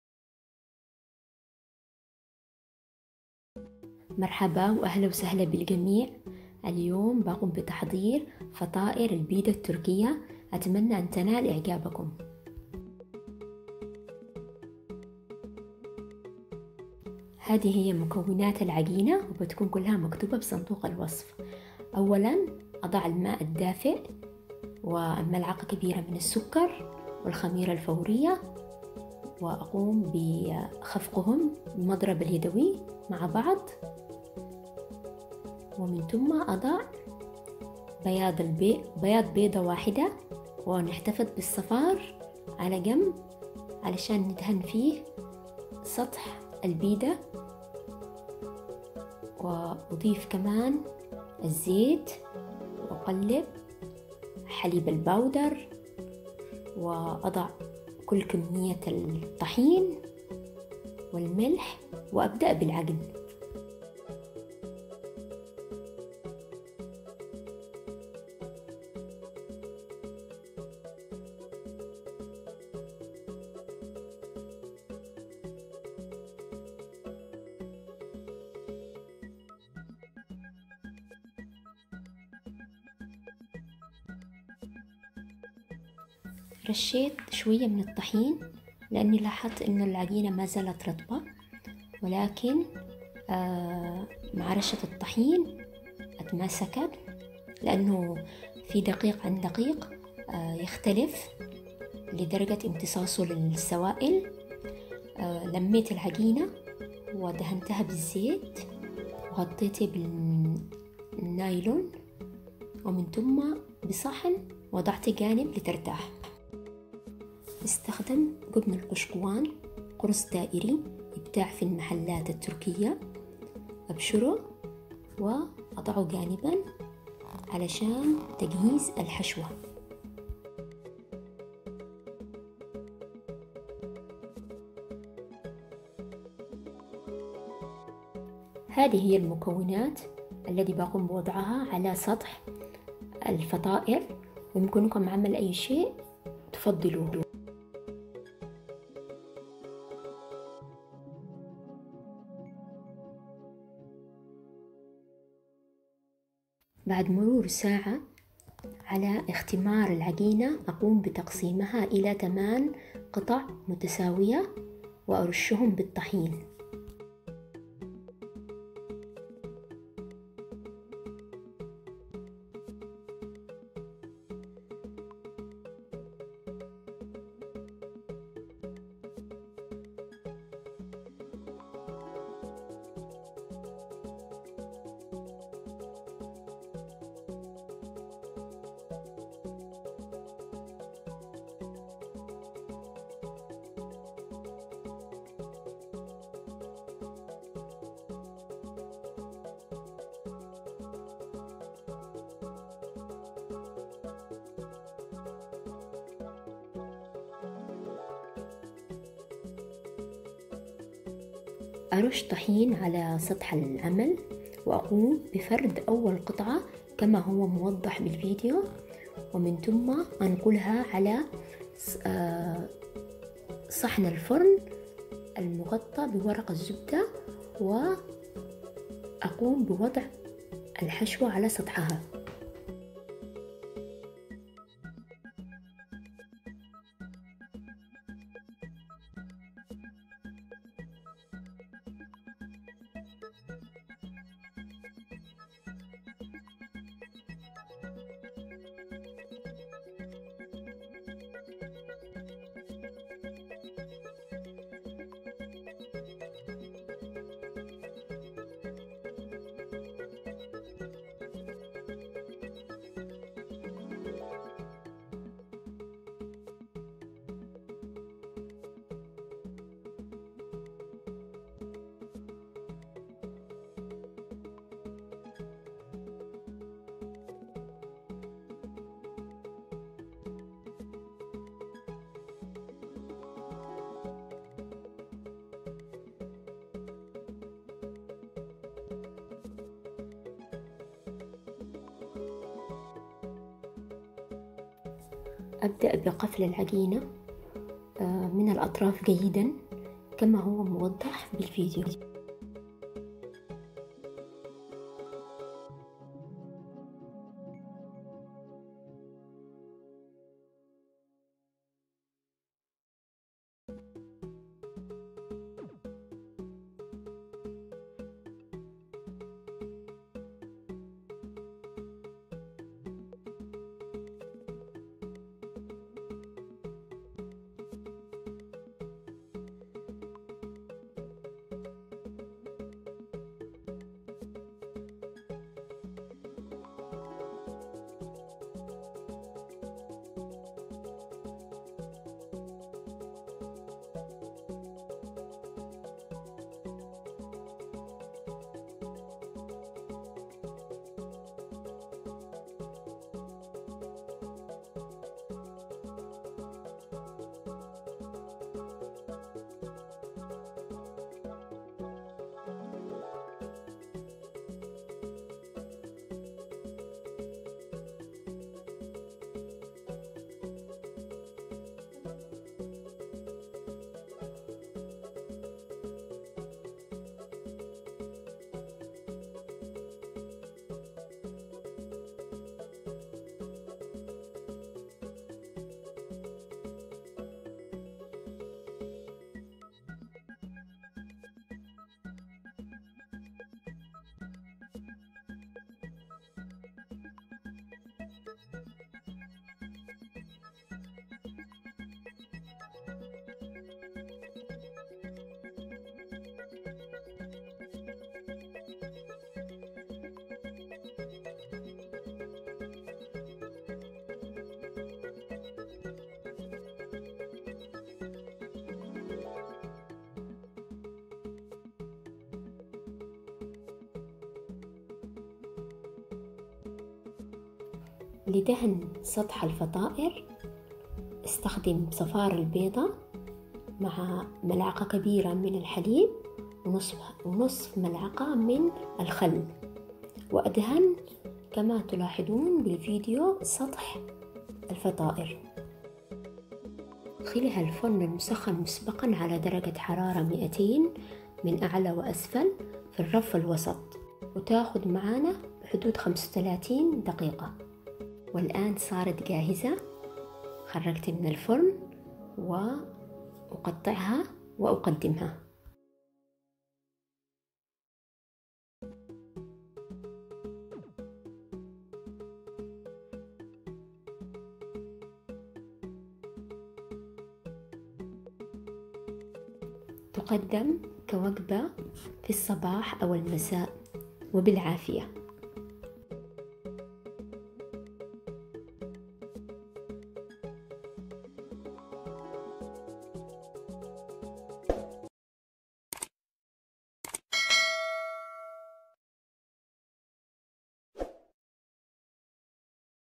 فطائر البيدا التركية
المكونات:
كوب ونص ماء دافئ.
ملعقة كبيرة من الخميرة.
ملعقة كبيرة من السكر.
ثلاث ملاعق كبيرة من الحليب البودرة.
بياض بيضة.
ربع كوب زيت.
أربعة أكواب طحين ابيض.
نصف ملعقة صغيرة من الملح.
الحشو حسب الرغبة
جبن القشقوان.
سجق و سلامي فلفل الوان.
لدهن سطح الفطائر:
صفار البيض +ملعقة ملعقة كبيرة من الحليب+ نص ملعقة صغيرة من الخل.